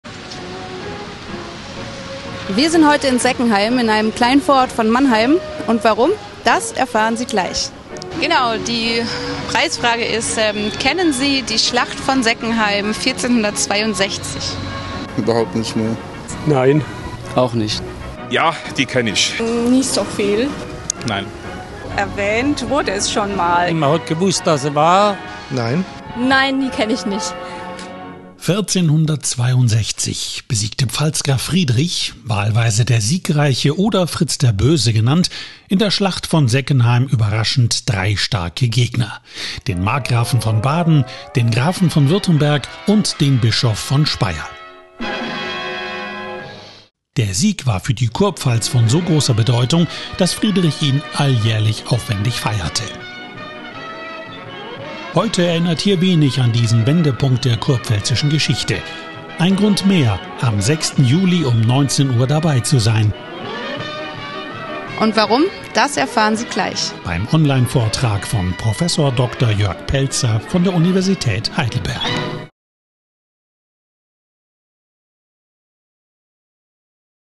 Die Schlacht von Seckenheim - Wer weiß noch davon? Trailer zur Vortragsreihe "Krieg und Frieden"
Am 30. Juni 1462 wurde auf dem Feld südöstlich des heutigen Seckenheim auf Leben und Tod gekämpft. Kurfürst Friedrich I. ging als Sieger aus der Schlacht hervor und ließ das Ereignis jährlich mit viel Aufwand in Heidelberg feiern.

Wie es zur Schlacht kam, und in welcher Form daran erinnert wurde, erläutert Prof. Dr. Jörg Peltzer von der Universität Heidelberg in seinem Vortrag "Vom Kampf auf Leben und Tod zur triumphalen Prozession".

Mittwoch, 06. Juli 2022, 19 Uhr
Eine Veranstaltung im Hinblick auf die Ausstellung "Krieg und Frieden - Konfliktarchäologie an Rhein und Neckar"

Videoproduktion: Lutz Berger, Doris Mayer, Bernd Bundschu